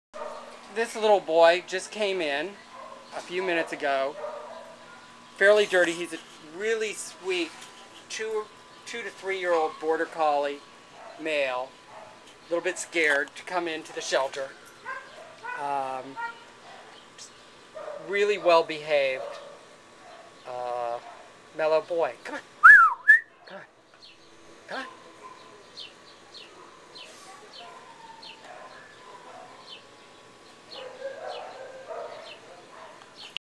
A3975068 Border Collie
This approximately two year old male border collie came into the Baldwin Park Shelter as a stray on May 19th and will be available for adoption on May 24th if his owner doesn't come claim him before then.  He is very friendly - but was quite filthy when found.